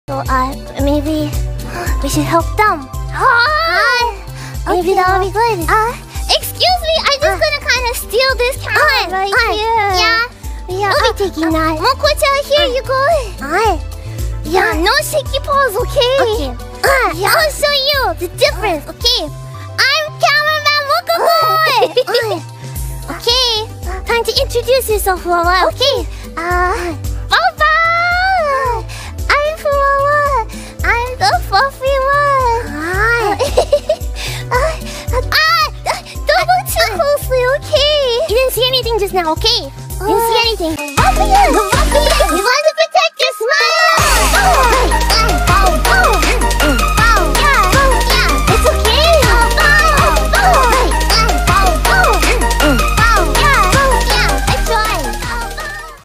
Mococo Being Camera Man Accidentally Shows Something
Mococo Taking Every Moment with Fuwawa she definitely knew what she doin Baubau!!🐾💕🐾💕